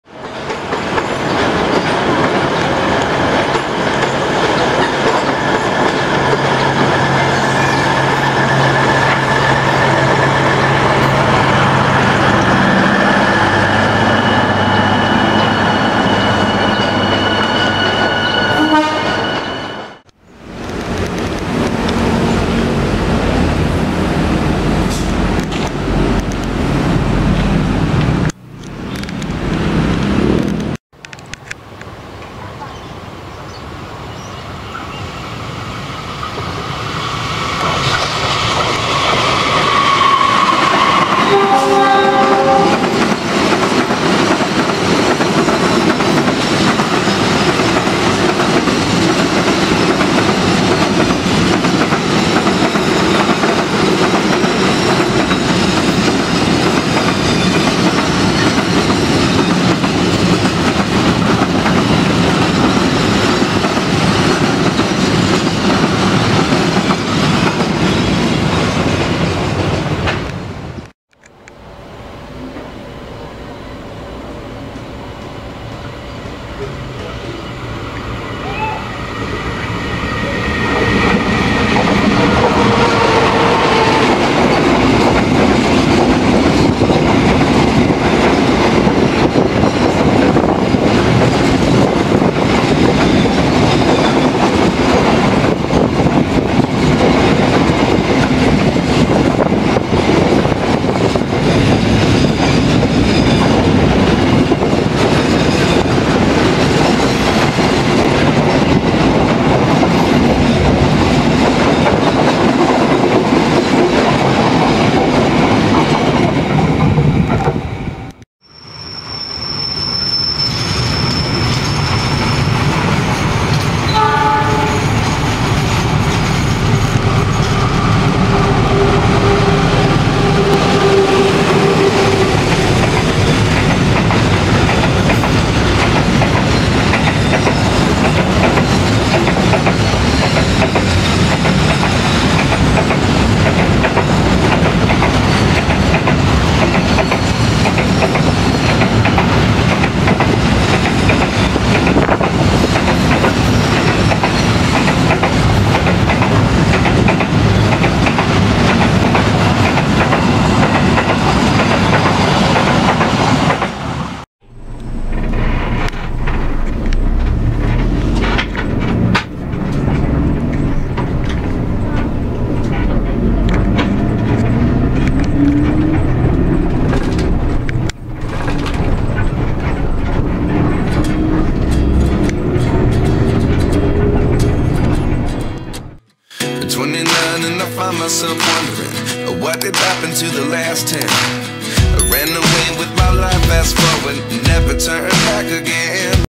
MENYAKSIKAN SALAH SATU KERETA API TERPANJANG DI PULAU JAWA...
Mengabadikan Singkat Salah Satu Kereta Api Terpanjang di Pulau Jawa...

Tonton Juga :
| PENAMPAKAN LOKOMOTIF LIVERY 2 GARIS BIRU VINTAGE PT KA DARI DEKAT...

| Mengejar Argo Parahyangan Panoramic Full Stiker di Ngebuburit H-1 Lebaran...

Hatur Nuhun Yang Sudah Menonton...🙏